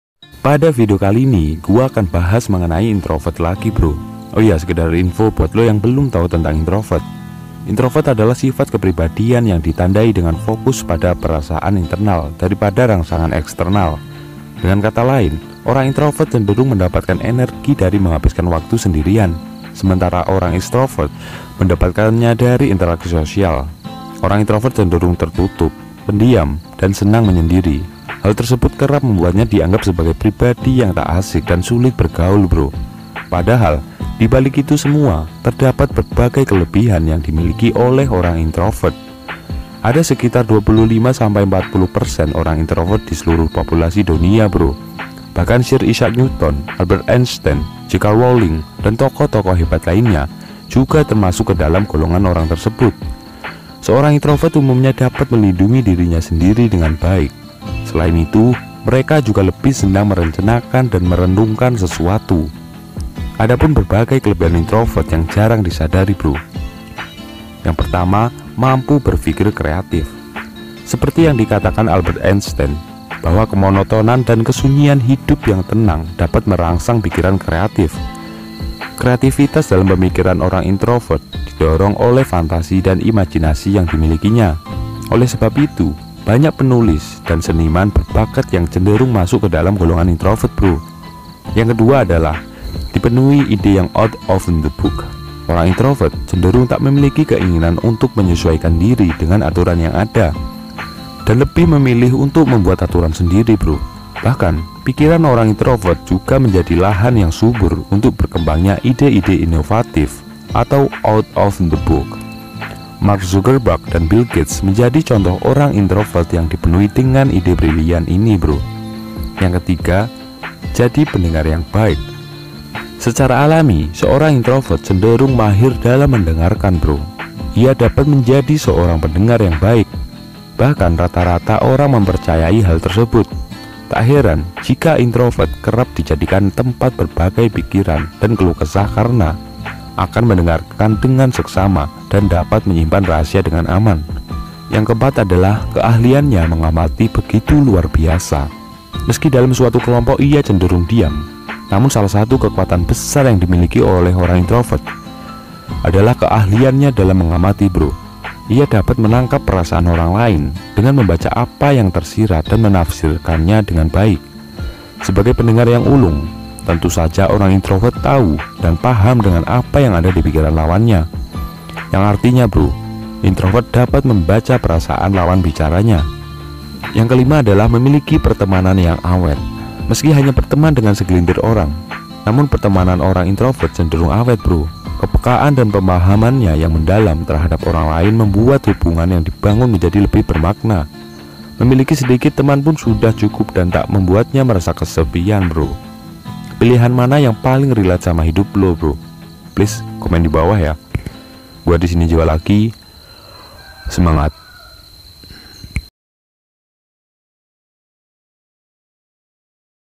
Introvert Punya Kelebihan Sepesial Yang Gak Dipunyai Semua Orang
Introvert Punya Kelebihan Sepesial Yang Gak Dipunyai Semua Orang @PriaSeratusPersen

Ini Kelebihan Jika Lo JADI Introvert Bro,kelebihan sepesial yang gak dipunyai semua orang,mungkin Lo gak sadar selama ini,ini kelebihanya Bro